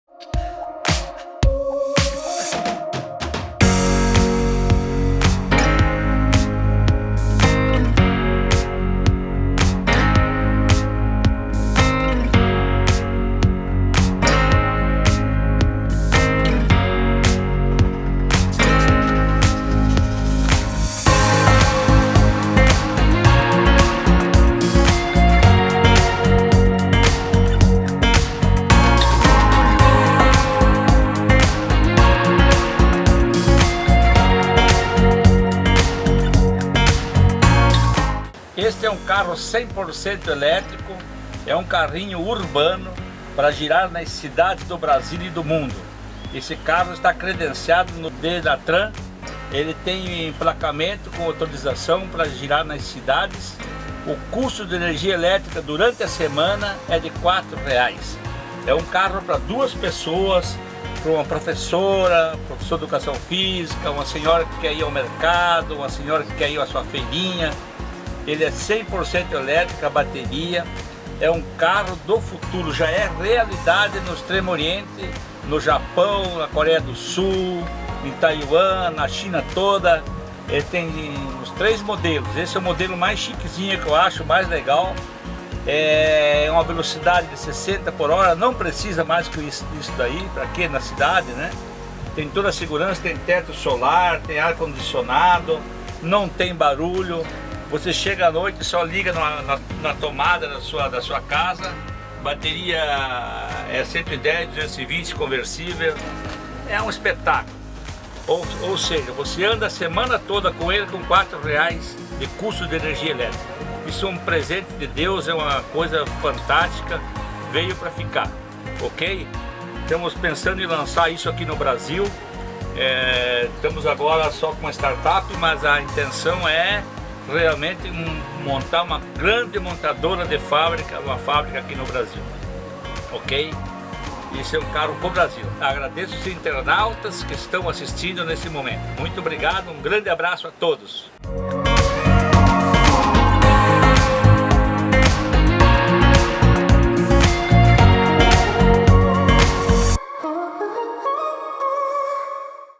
Carro elétrico já é realidade no Brasil
Dr. Jovelci Gomes, Presidente do Grupo Sul Brasil apresenta um carro que promete revolucionar o mercado brasileiro.